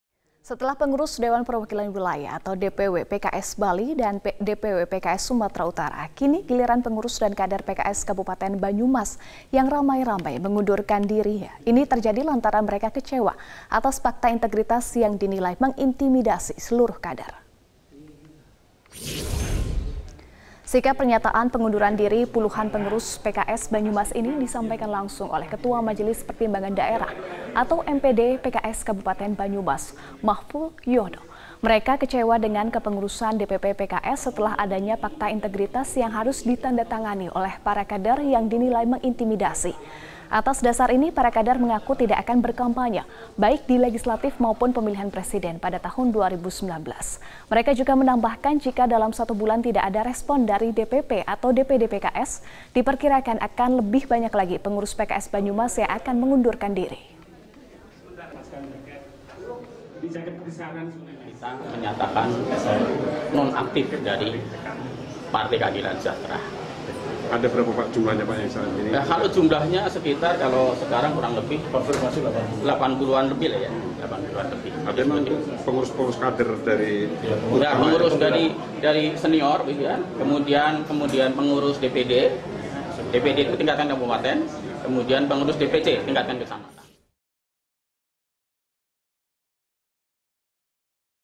Pengurus dan Kader PKS di Banyumas Kompak Mundur
Setelah pengurus Dewan Perwakilan Wilayah (DPW) PKS Bali dan Sumut, kini giliran pengurus dan kader PKS Kabupaten Banyumas yang ramai-ramai mengundurkan diri. Hal ini terjadi lantaran kecewa atas pakta integritas yang dinilai mengintimidasi seluruh kader.